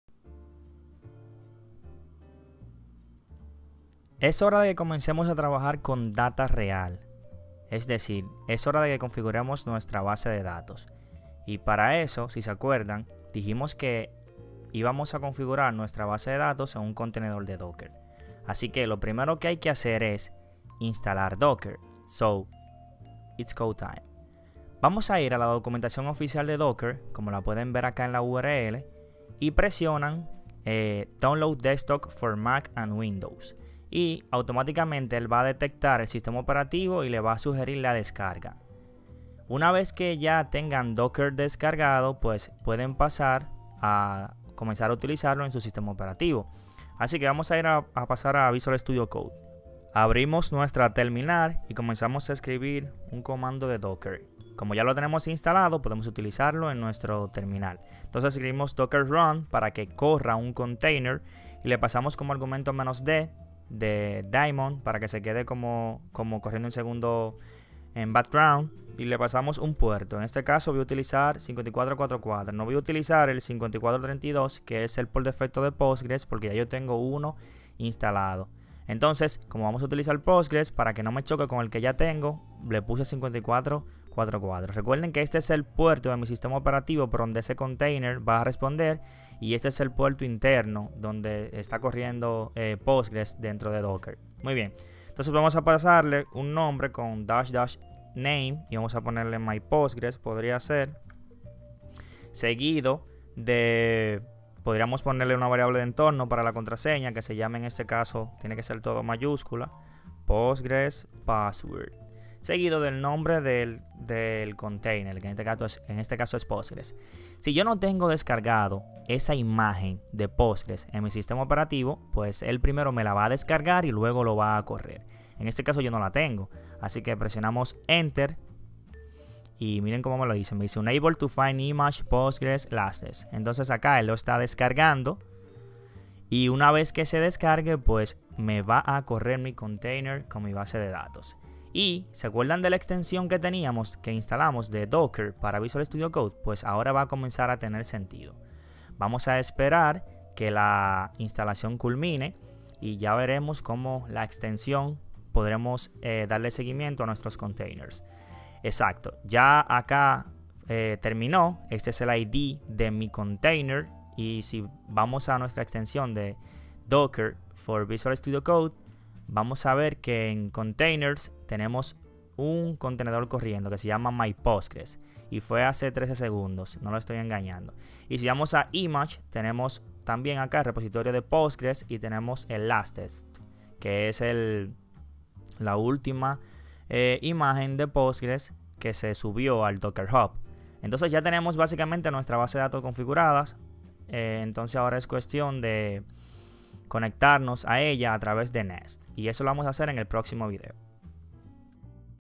014. Nest.js Series: Creando container de Postgres
Montar una base de datos Postgres nunca había sido tan fácil. Con Docker mucho de nuestros problemas han sido solucionado. Así que vamos a aprovechar la magia de los contenedores para montar uno que nos provea una base de datos Postgres. En este video te enseño cómo hacerlo.